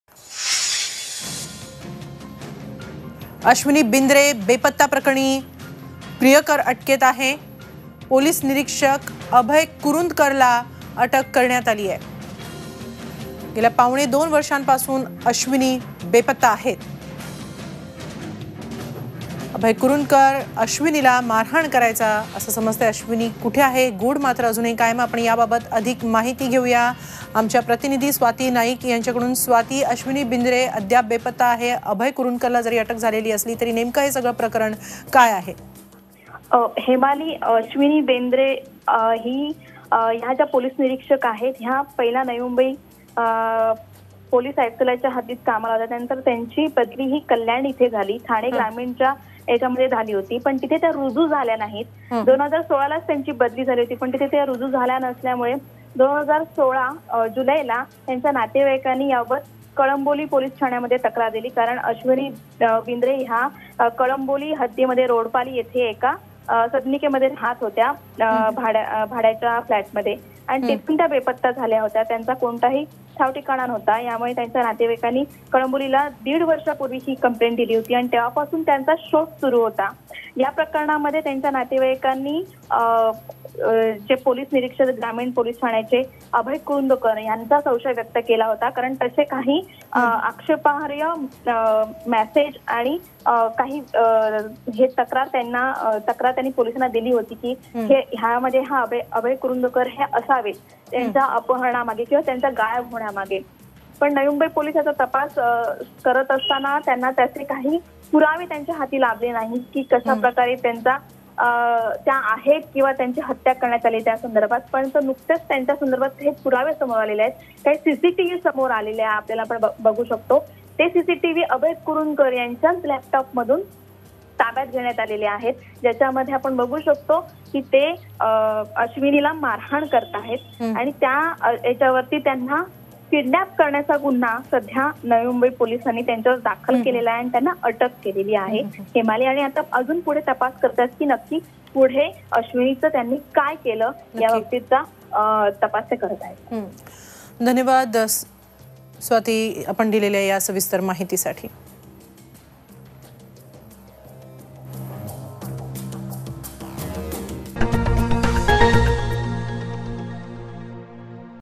नवी मुंबई | आश्विनी बिंद्रे बेपत्ता प्रकरणी प्रियकर अटकेत
Navi Mumbai | Police Inspector | Abhay Kurundakar Arrested